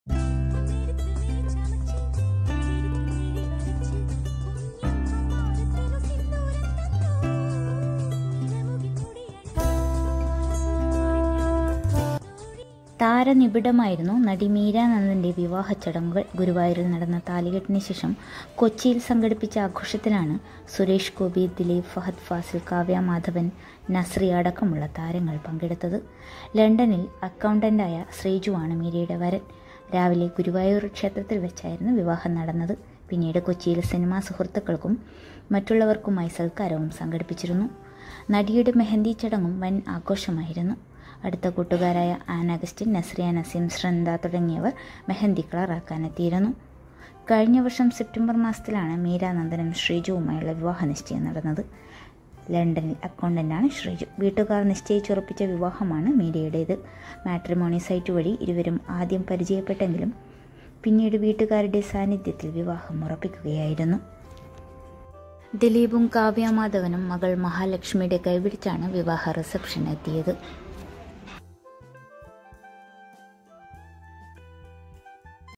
മീര നന്ദന് ആശംസകളുമായി ഫഹദും ദിലീപും കാവ്യയും; ശ്രദ്ധ നേടി വിഡിയോ...
കൊച്ചിയില്‍ സംഘടിപ്പിച്ച ആഘോഷത്തിലാണ് സുരേഷ് ഗോപി, ദിലീപ്, ഫഹദ് ഫാസില്‍, കാവ്യ മാധവന്‍ അടക്കമുള്ള താരങ്ങൾ പങ്കെടുത്തത്. ...